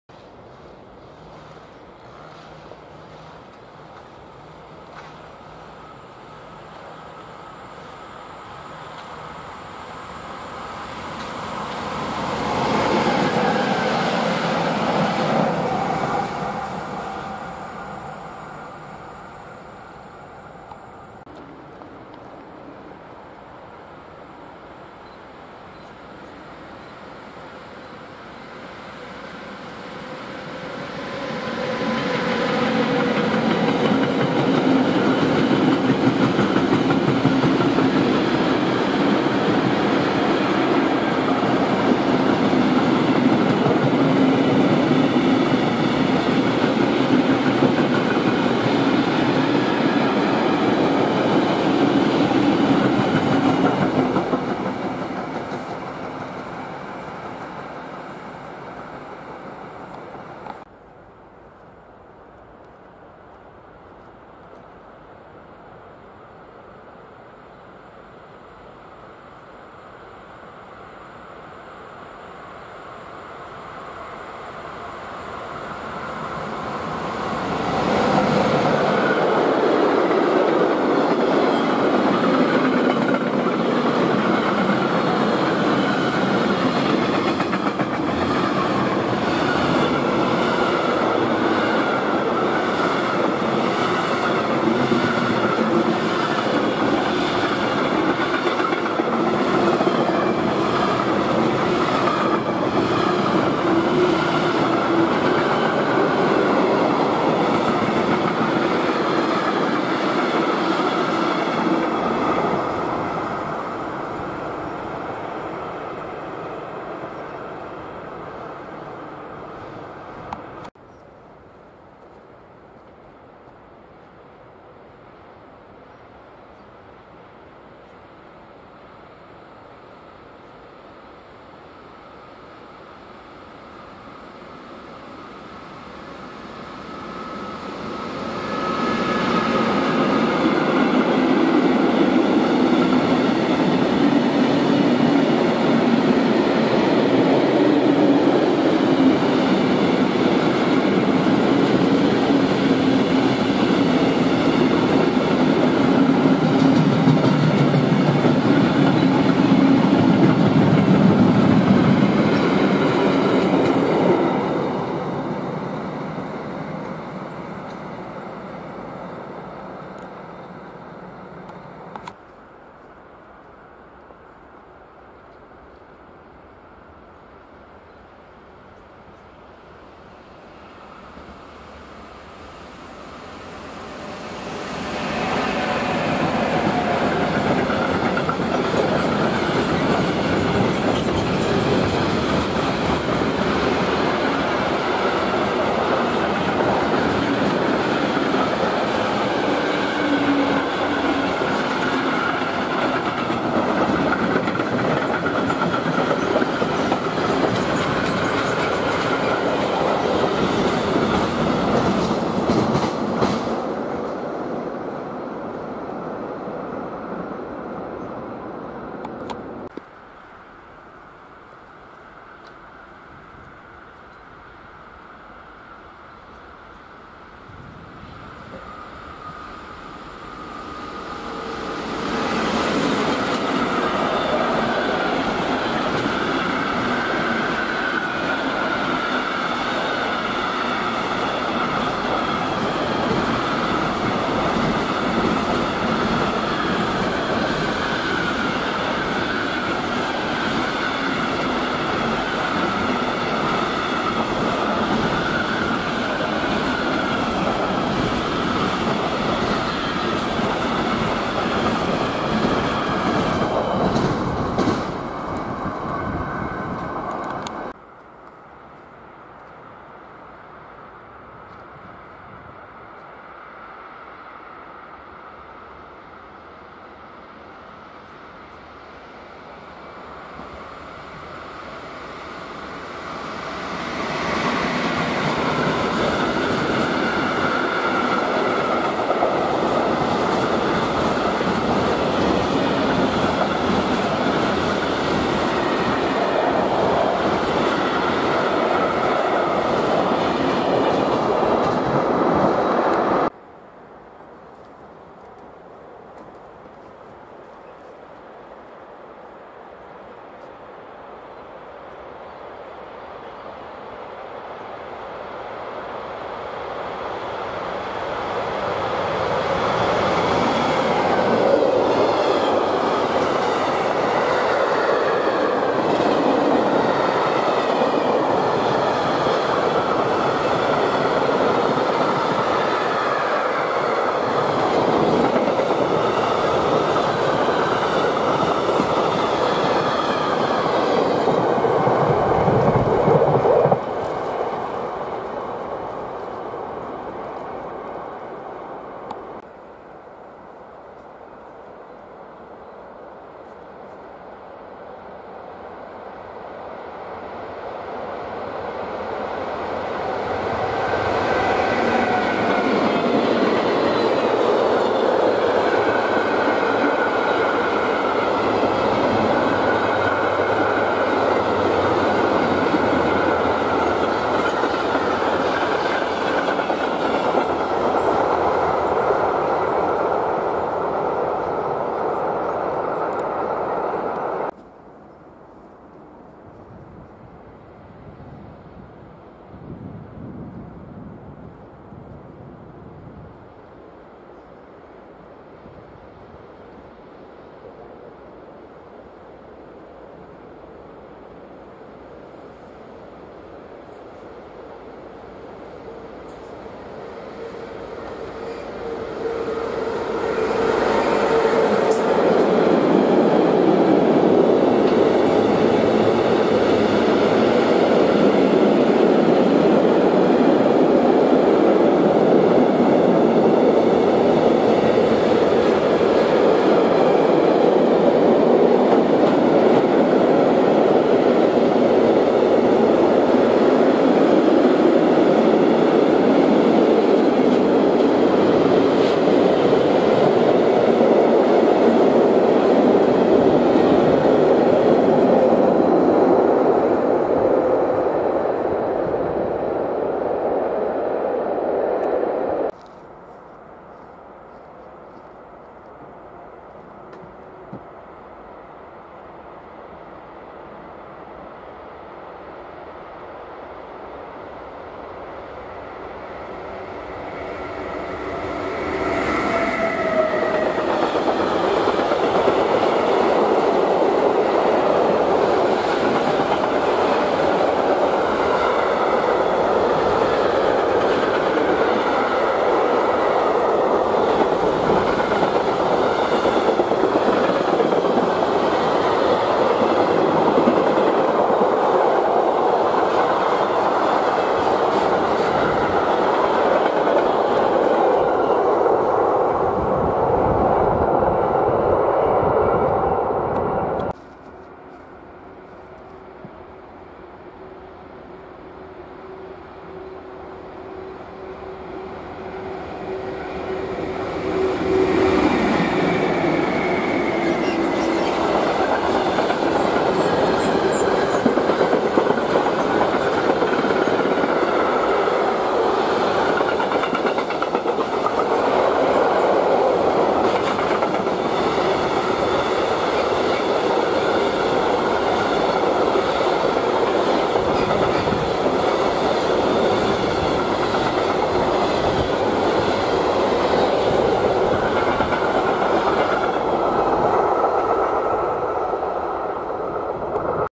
Sichtungen am 26.11.2019 teil 1 / DB Cargo,MRCE,Rail-Pool.
Zugverkehr  in Nordrhein-Westfalen
0:00   Br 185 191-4 + Br 185 ? + Br 185 ? DB Cargo
0:21   Br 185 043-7 DB Cargo
1:00   Br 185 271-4 DB Cargo
2:00   Br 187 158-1 DB Cargo
2:52   Br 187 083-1 + Br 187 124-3 DB Cargo
3:39   Br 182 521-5 MRCE
4:28   Br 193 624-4 MRCE
5:03   Br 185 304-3 DB Cargo
5:49   Br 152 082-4 DB Cargo
6:24   Br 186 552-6 Rail-Pool
7:32   Br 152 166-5 DB Cargo
8:24   Br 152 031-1 + Br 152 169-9 DB Cargo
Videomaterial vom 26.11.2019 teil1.

Zapraszam na  kolejny film na moim  kanale !)
Pociągi  w  Nadreni Północnej-Westfali  ( Niemcy )
Filmy nagrane w dniu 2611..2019 cz1.
Jeżeli się podobało to daj łapkę,  lub zostaw suba
Pozdrawiam wszystkich Miłośników kolei,
oraz odwiedzających moją strone na YT